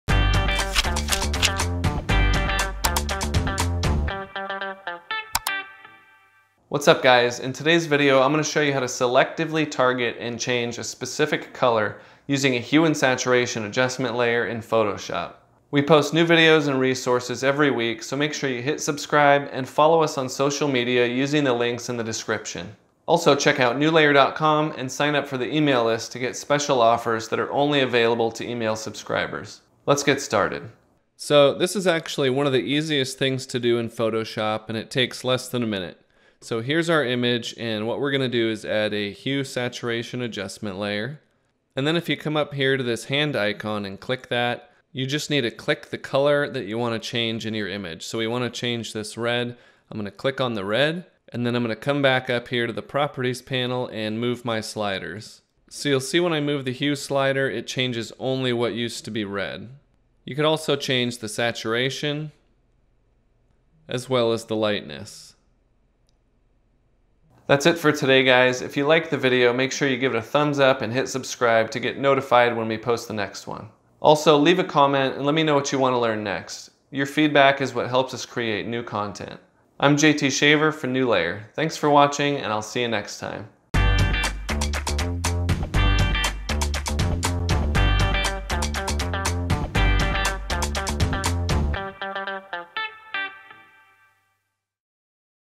Target a Specific Color with a Hue and Saturation Adjustment Layer in Photoshop
Changing specific colors in an image used to require masking and other time consuming tricks. Nowadays, you can target and change individual colors with ease.

This tutorial will show you how to use one special button within a Hue and Saturation Adjustment Layer to change a specific color in seconds.

You can use this trick to change the color of clothing, backgrounds, and other objects quickly and easily.

Members like you make it possible to create new content every week.

== DOWNLOADS ==

== CONNECT ==